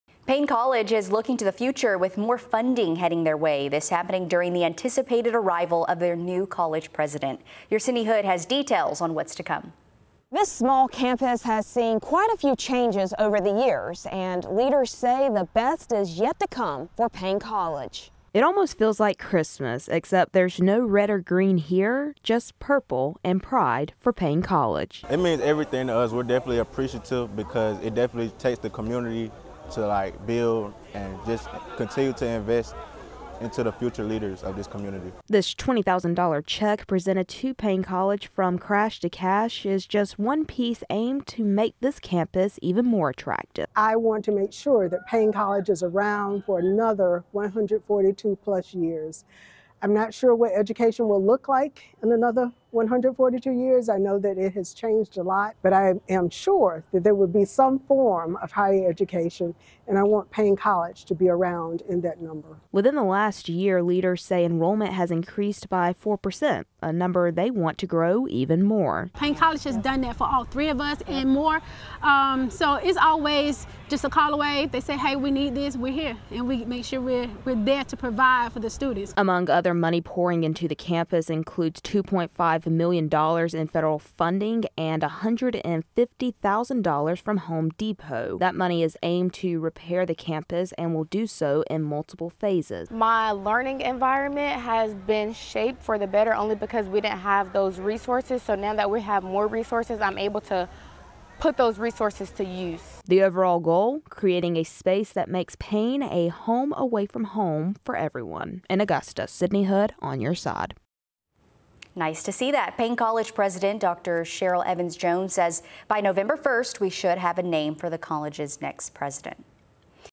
Paine College sees more changes due to new funding
Paine College is looking to the future with more funding heading their way. Plus, the anticipated arrival of their new college president.